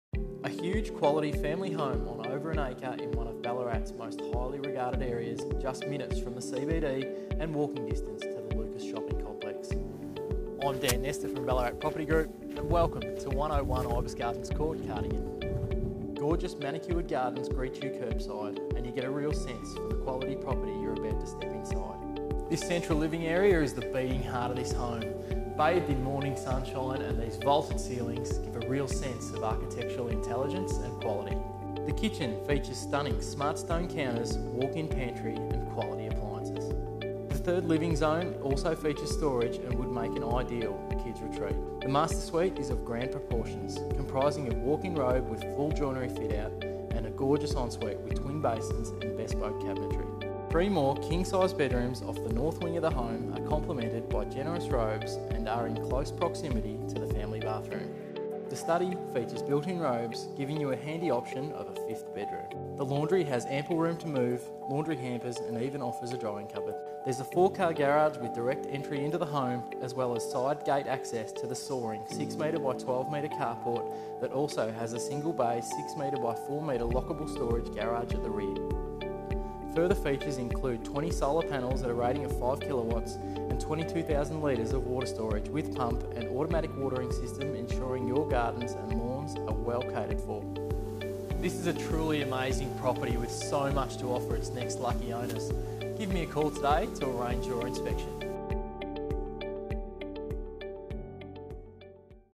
Daniel Nestor from Ballarat Property Group proudly presents 101 Ibis Gardens Court, Ballarat
A masterful blend of architectural design, modern luxuries and conveniences within a few minutes of the Ballarat CBD.
Ballarat Property Group is excited to present to you Winterose. Situated proudly in the much hyped Ibis Gardens estate, in the suburb of Cardigan, just on the outskirts of Alfredton and a short stroll to the brand new Lucas Shopping Centre. This expansive home is complimented by a private and peaceful one acre allotment.
Upon entry youll be impressed by the striking bamboo flooring, so hardy yet screaming quality. Formal sitting room overlooks the thoughtfully designed and precisely maintained front gardens and is adjacent the study - with built  in robe  making this a 5th bedroom option if required. The central living zone is the hero of the home  bathed in morning sunlight, and the vaulted ceiling gives a real sense of architectural intelligence and quality. The kitchen features stunning smart stone counters, walk in pantry and quality appliances. A third living zone also features storage, and would make an ideal kids retreat  or perhaps a rumpus ideal for a championship snooker table. The master suite is just fabulous. Of grand proportions, comprising walk in robe with full joinery fitout, and a gorgeous ensuite with twin basins and bespoke cabinetry. 3 more king size bedrooms off the north wing of the home are complimented by generous robes and are in close proximity to the family bathroom. The laundry has ample room to move, laundry hampers and drying cupboard.
There is direct access to the 4 car garage and rear roller access to this area also. Side access to the rear leads you to the 6m x 12m soaring carport with single 6m x 4m lockable storage at the rear (roller door and entry door access). 20 solar panels line carport/shed roof at a rating of 5kw  keeping your bills at a minimum all year round.
22000 litres of water storage with pump ensures the gardens and lawns surrounding the home are never thirsty! So much to mention  so much to show you. Book your inspection at 101 Ibis Gardens Court today.